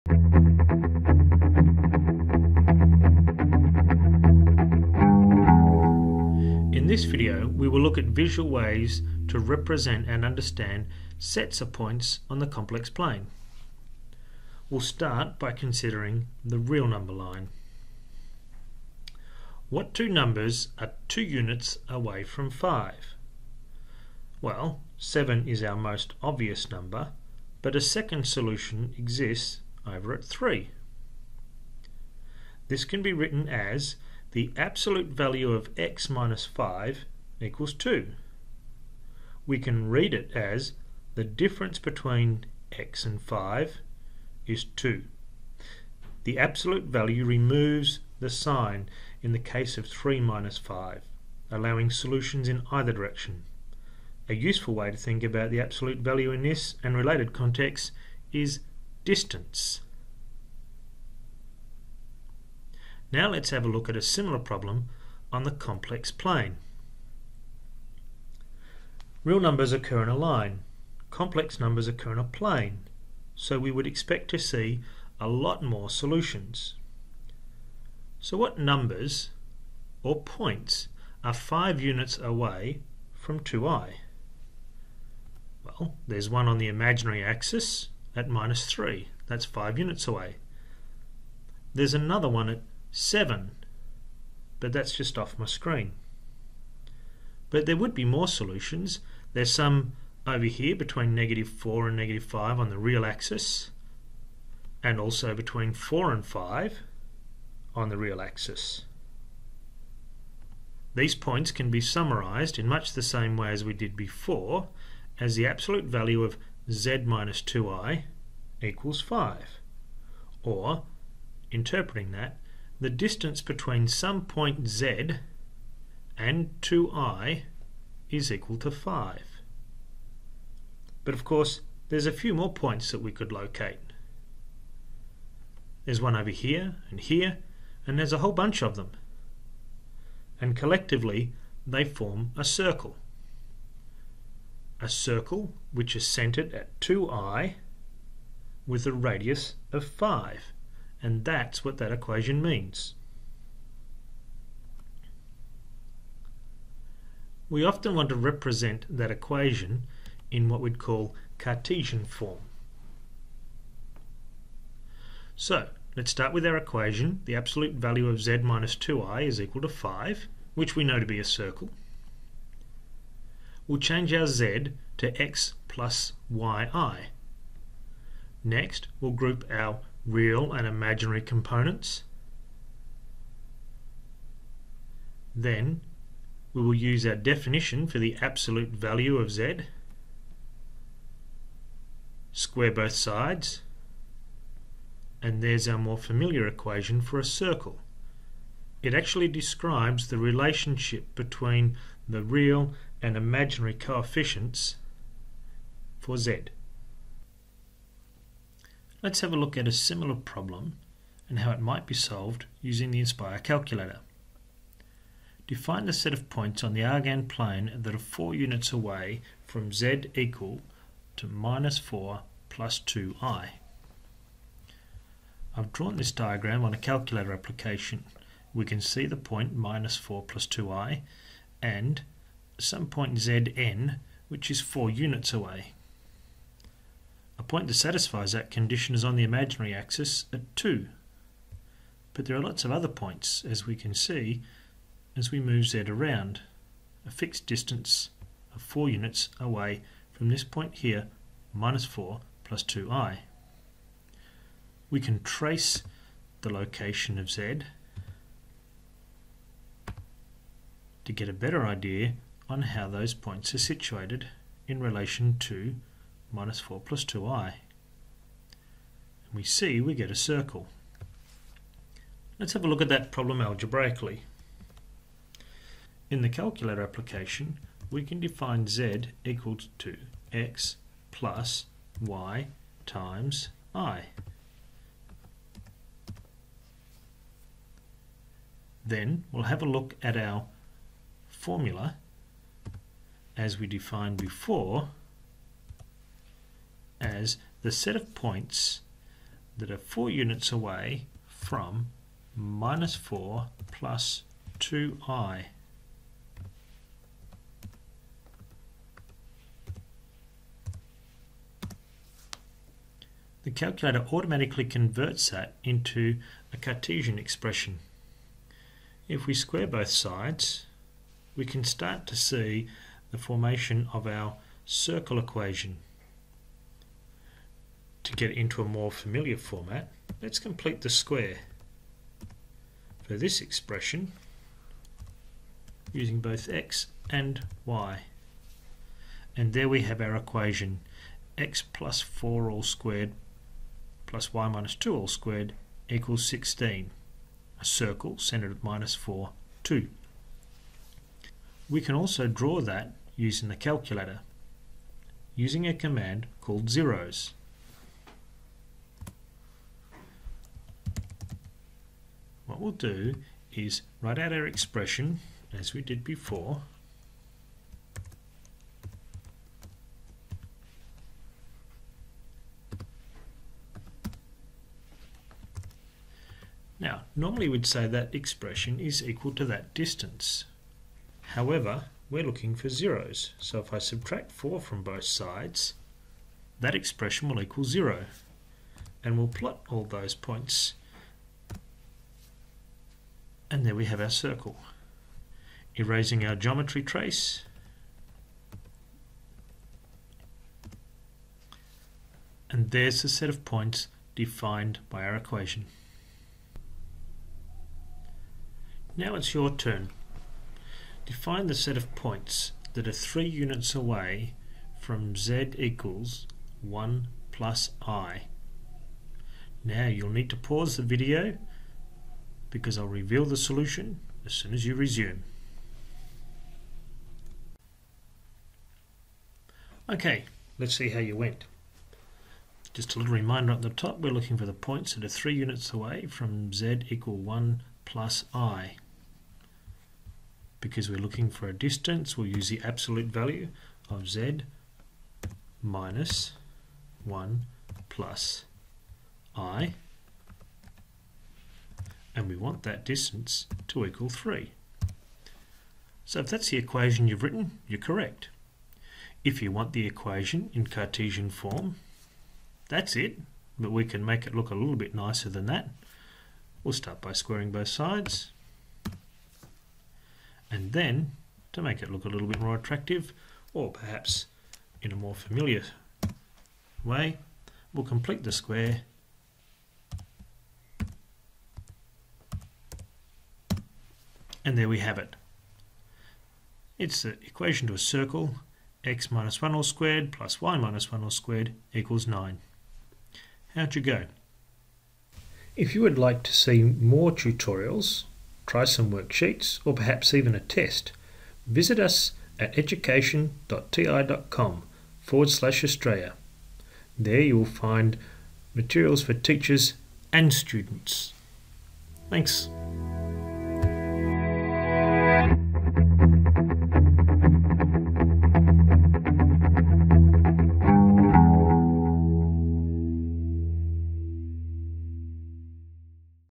Circle on the Complex Plane
How do you know what sort of graph will be created from a complex equation? This video provides worked examples and builds the concepts required to recognise equations and graphs of complex numbers on the Argand Plane